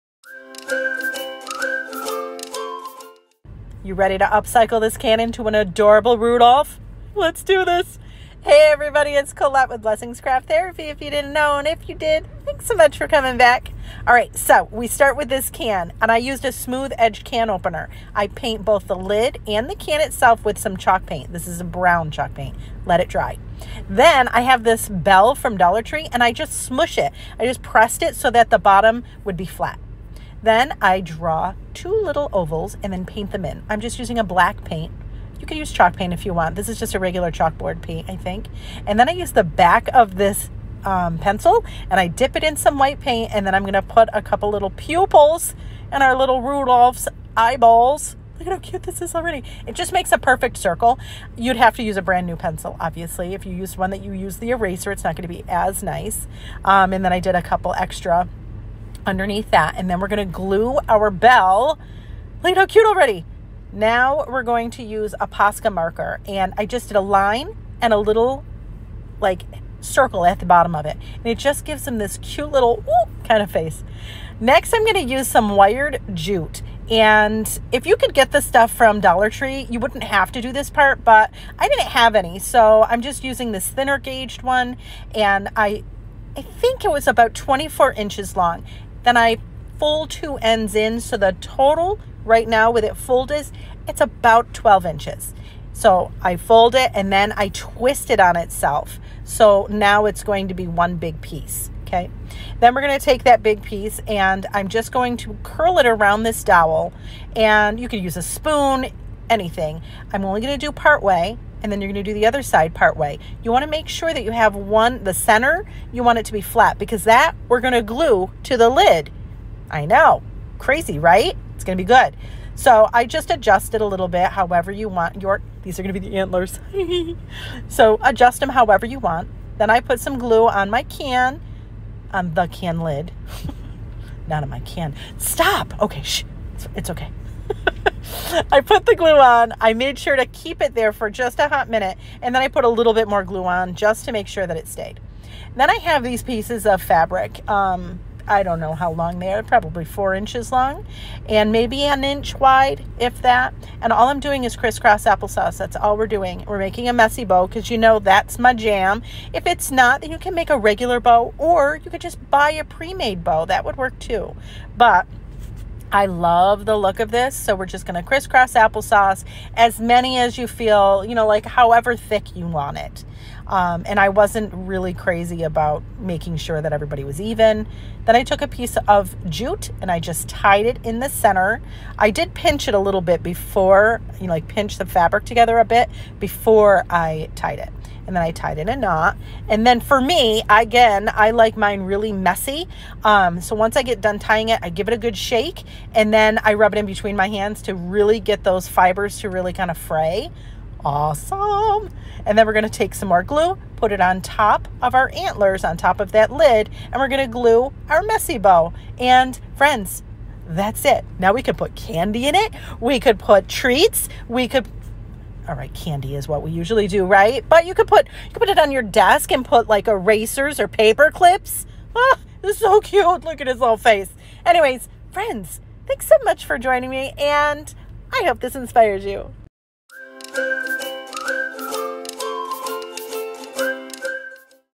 Let's upcycle a tin can & Diy Tin Can Rudolph
Let's upcycle a tin can &  Diy Tin Can Rudolph! This is came out crazy pants cute!
Know how much I appreciate you all!  Y'all make me smile big! ❤️